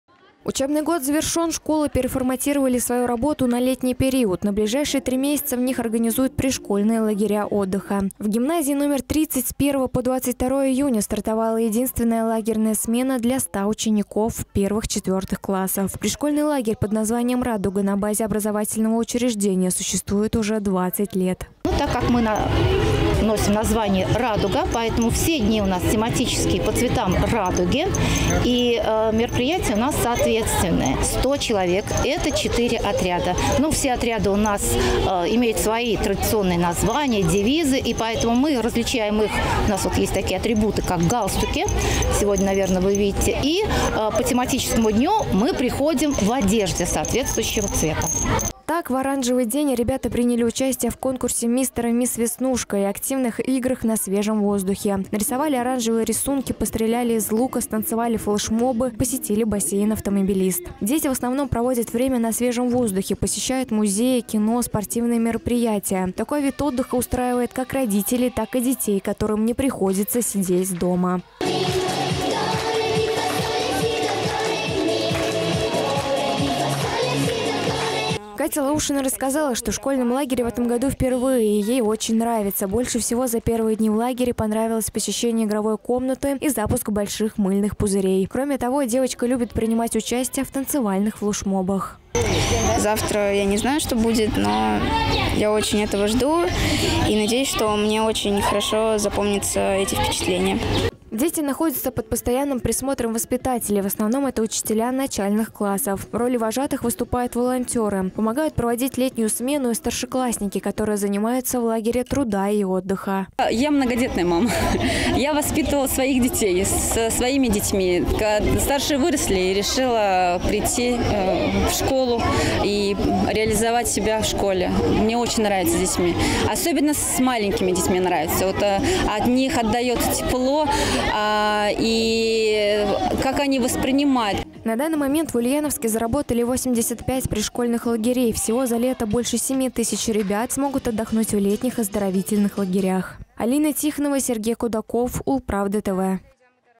Бассейн, веснушки и оранжевый день. Как дети отдыхают в пришкольных лагерях
Летние каникулы начались, свои двери открыли детские лагеря. Для тех, кто не готов расставаться с детьми надолго, заработали дневные смены при школах. Чем занимаются ульяновские ребята летом – смотрите сами.